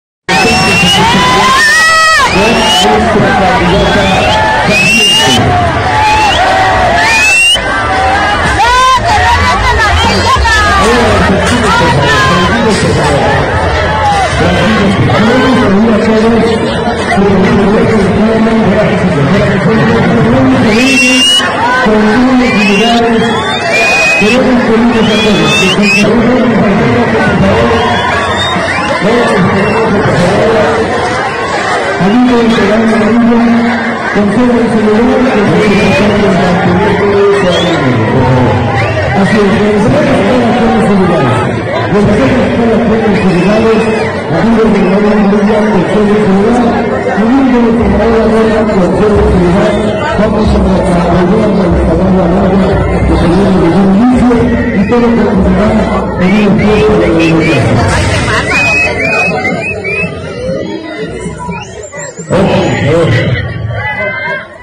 Militantes del PRI y PRD se agarran a sillazos en mitin de Alejandra del Moral
A pesar de que los separaban vallas metálicas, militantes del PRI y PRD se enfrentaron en Toluca en el cierre de campaña de la priista en la Plaza de los Mártires